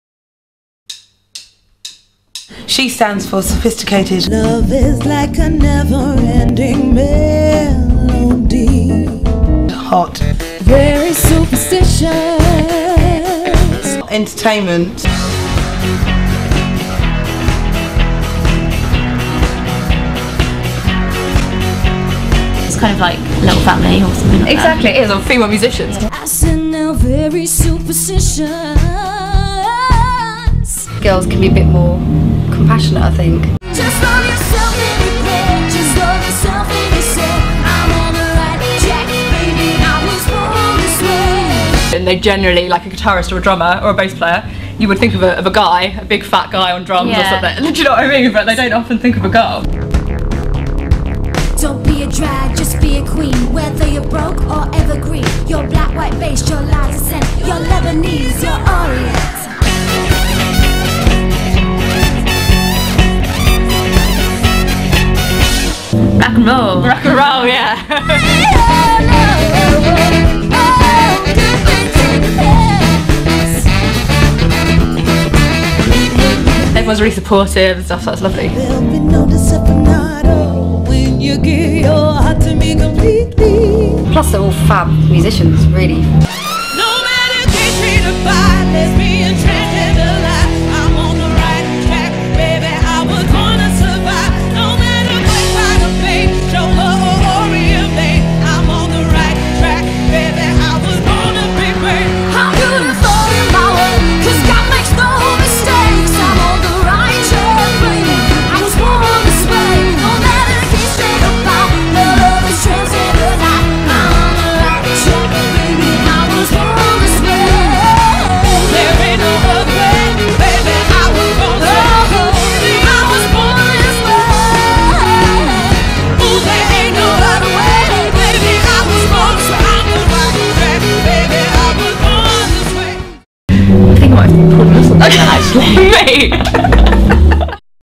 S.H.E! Band
S.H.E! (Sophisticated Hot Entertainment) is the UK's number 1 all female function band. They truly are a wonderful addition to any party or event. This trailer gives you a smidgen of an insight in to these talented Ladies.